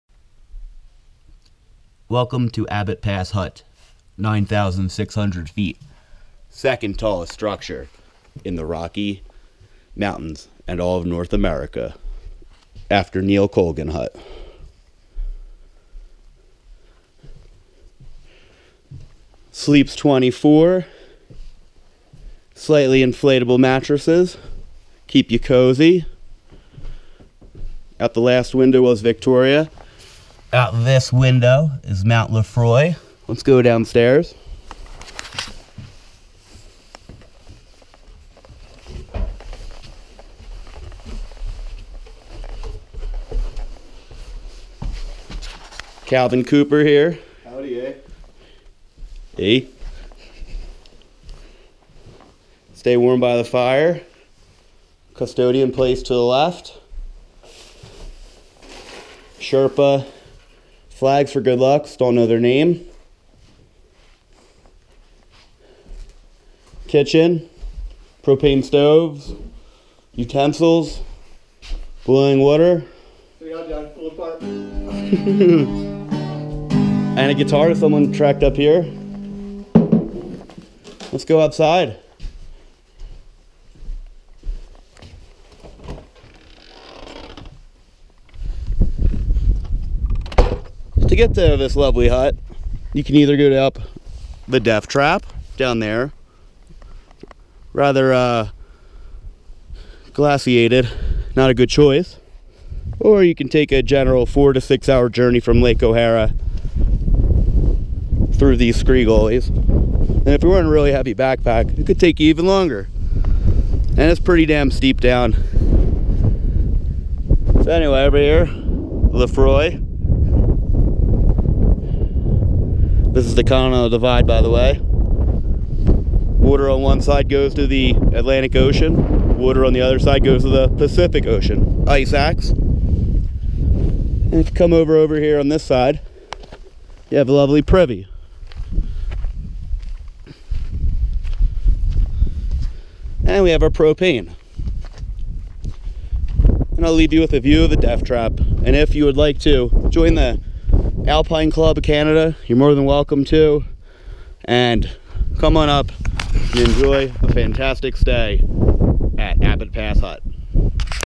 Welcome to Abbot Pass Hut
The Abbot Pass hut is an alpine hut located at an altitude of 2925 metres (9,598 feet) in Abbot Pass in the Rocky Mountains in Alberta, Canada. It is nestled between Mount Victoria and Mount Lefroy, straddling the continental divide, which, in this region, defines the boundary between Banff National Park in Alberta and Yoho National Park in British Columbia. While close to the border, the hut lies entirely in Banff National Park, and is the second-highest permanently habitable structure in Canada (after the Neil Colgan Hut). The hut is maintained by the Alpine Club of Canada.